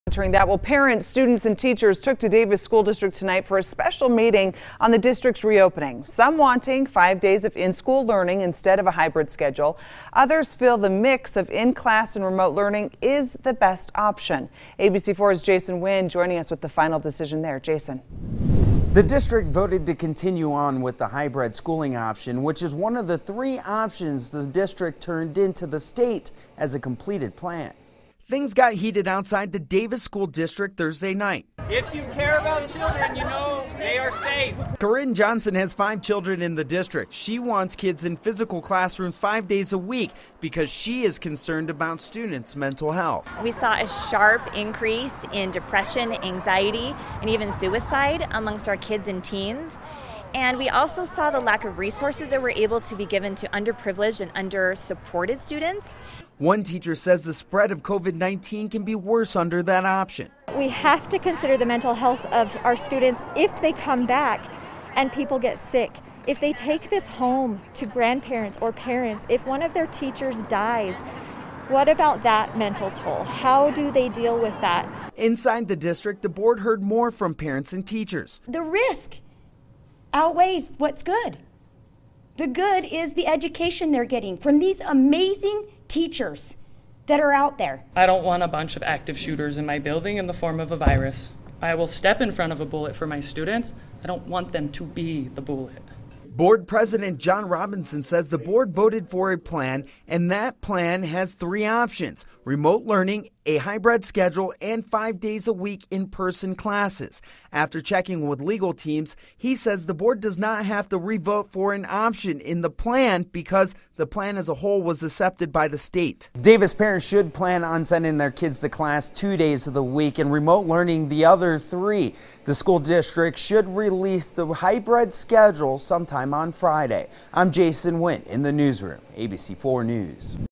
Davis School District moves forward with the hybrid option
The Davis School District voted unanimously to continue with the hybrid school option.
Which is one of three options the board turned into the state as a complete plan.